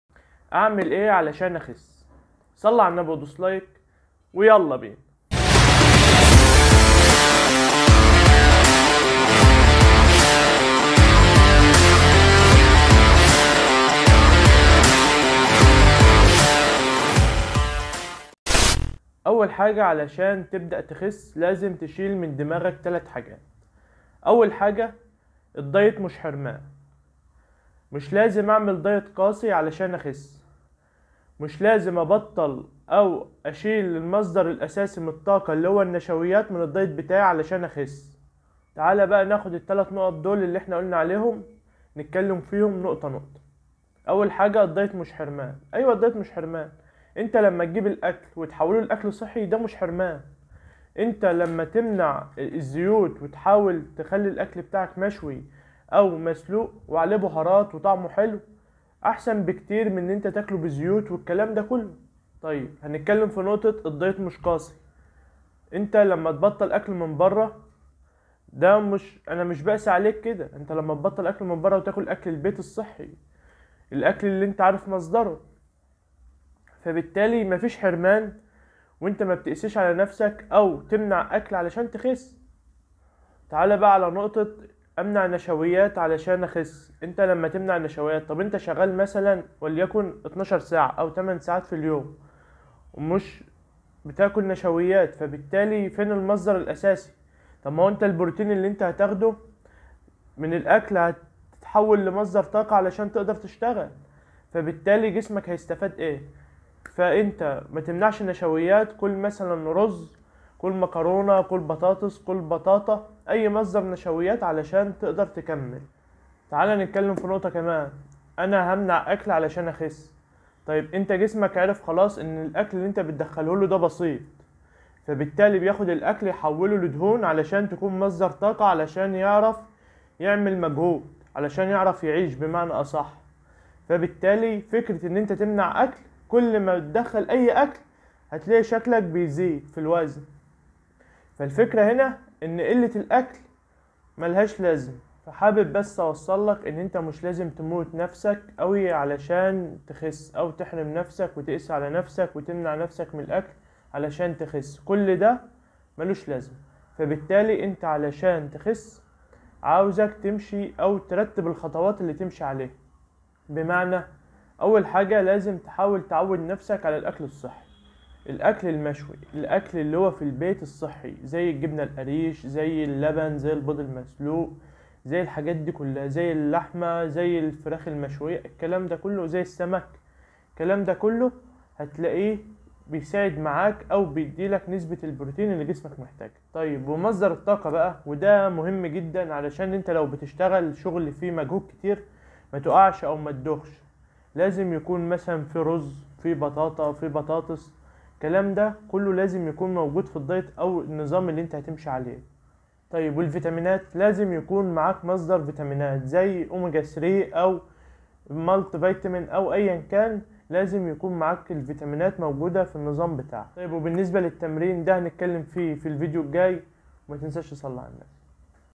تعمل أي لو مش عارف تخس ؟
عشان تخس بتفضل تفكر كتير وتحط حاجات في دماغك بتوقفك تعاله نشوف الحاجات دي في الفيديو